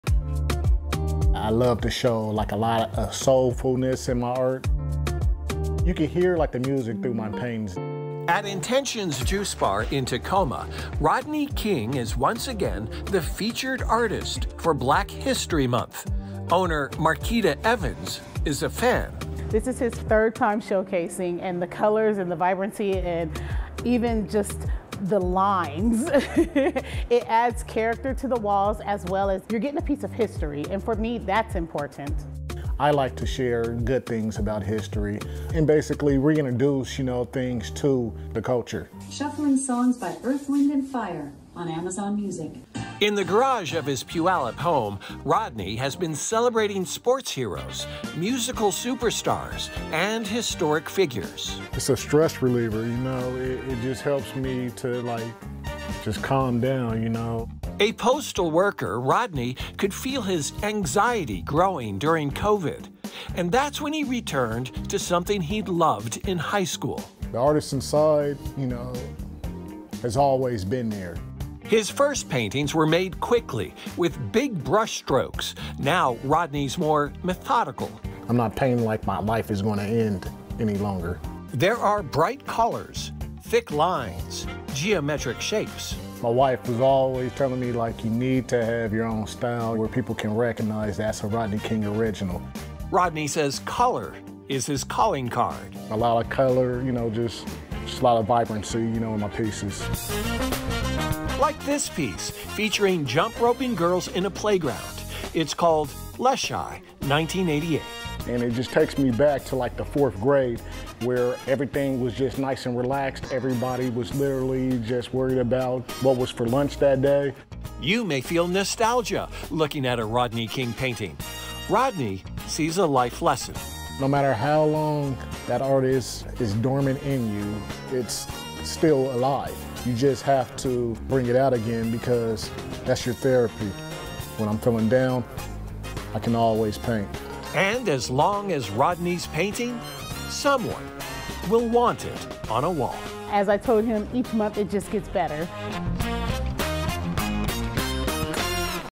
King of Paint: Puyallup artist Rodney King paints Black history in vivid colors
At Intentions Juice and Smoothie Bar in Tacoma, Rodney King has once again been the featured artist for Black History month. Owner Marquita Evans is a fan.

"This is his third time I've been showcasing his art and I love the colors and the vibrancy and even just the lines," Evans said. "It adds character to the walls as well as you're getting a piece of history and, for me, that's important."

"I like to share the good things about history and basically reintroduce things to the culture," King said.

In the garage of his Puyallup home, King streams old school soul as he sits down to paint. His subject matter might be sports heroes like Jerry Rice and Florence Griffith-Joyner or music icons like Marvin Gaye and Aretha Franklin.

"I love to show like a lot of soulfulness in my art," King said. "You can hear the music through my paintings."

There's a painting of Martin Luther King Jr. giving the "I Have a Dream" speech and another one featuring the Harlem club boxer Sugar Ray Robinson.

King could feel his anxiety growing during the pandemic. He works for the post office, so working from home was never an option. There was a sameness to every day that further irritated King. That's why he returned to something he loved in high school.

"It's a stress reliever," King said. "It just helps me to calm down, you know?"

His first paintings were made quickly with big brush strokes. Now King's more methodical.

"I'm not painting like my life is gonna end any longer," he said.

There are bright colors, thick lines, and geometric shapes.

"My wife was always telling me, 'you need to have your own style where people can recognize that's a Rodney King original,'" King said.

Color is his calling card.

"A lot of color," he said. "Just a lot of vibrancy in my pieces."

There's a painting featuring jump roping schoolgirls called "Leschi, 1988."

"It just takes me back to the fourth grade where everybody was literally just worried about what was for lunch that day," he said.

You may feel nostalgia looking at a Rodney King painting. He sees a life lesson.

"No matter how long that artist is dormant inside you, it's still alive," King said. "You just have to bring it out again because that's your therapy. When I'm feeling down I can always paint."

And as long as Rodney King is painting there's someone like Marquita Evans who is interested in putting it on a wall.

 "As I told him each month it just gets better," Evans said.

To see Rodney King's paintings you can visit Base Camp Studios in Belltown on March 10th from 6-9 p.m. He'll also be at the Tacoma Art Market on March 12th located in Tacoma at 611 S. Baker St. from noon to six. Finally, you can his works at Pudding and Paint at the Tukwila Community Center on March 17th from 8-10:45 p.m